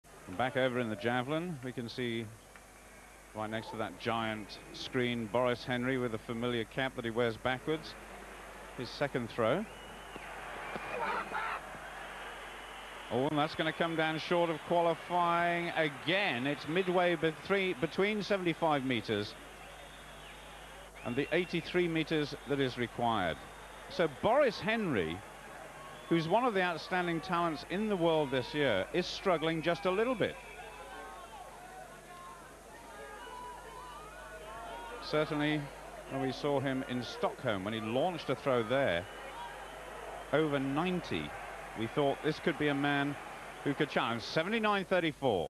6998 World Track and Field 1997 Javelin Men Boris Henry
1997 World Athens Javelin Men Qualifying Boris Henry 79m34
Buying something on Amazon
1 2  Boris Henry (GER) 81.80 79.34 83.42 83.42 m
2 3  Kostas Gatsioudis (GRE) 83.32 — — 83.32 m
3 4  Mick Hill (GBR) X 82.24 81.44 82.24 m
4 5  Gregor Högler (AUT) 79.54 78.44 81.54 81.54 m
5 8  Emeterio González (CUB) X X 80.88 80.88 m
6 10  Marius Corbett (RSA) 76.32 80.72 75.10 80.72 m
7 11  Sergey Makarov (RUS) X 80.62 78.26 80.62 m
8 13  Raymond Hecht (GER) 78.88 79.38 X 79.38 m
9 15  Tom Pukstys (USA) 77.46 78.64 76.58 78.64 m
10 18  Sami Saksio (FIN) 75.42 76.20 X 76.20 m
11 20  Pål Arne Fagernes (NOR) 75.66 X X 75.66 m
12 22  Ēriks Rags (LAT) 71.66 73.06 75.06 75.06 m
13 24  Roald Bradstock (USA) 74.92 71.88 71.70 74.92 m
14 26  Nick Nieland (GBR) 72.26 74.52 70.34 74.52 m
15 28  Firas Al-Mahameed (SYR) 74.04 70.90 X 74.04 m
16 29  Edgar Baumann (PAR) X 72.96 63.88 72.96 m
17 33  Aleksandr Fingert (ISR) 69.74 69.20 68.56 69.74 m
18 37  Robert Tersek (SLO) X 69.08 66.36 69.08 m
19 39  Vladimir Parfyonov (UZB) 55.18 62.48 X 62.48 m
20 40  Ponsianus Gnemen (INA) X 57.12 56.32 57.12 m